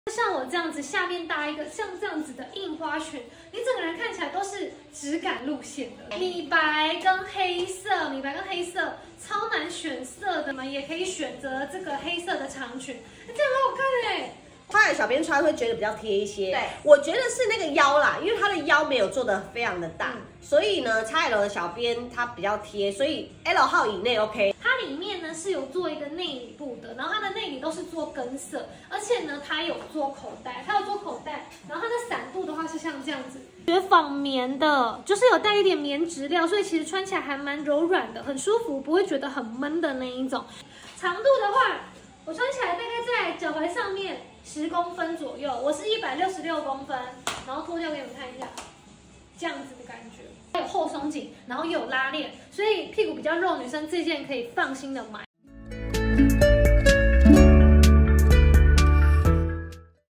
HARPER 後鬆緊側拉鍊水墨印花長裙 3色 -小柔+家琪穿搭示範
IG粉絲專頁👉harper_officialtw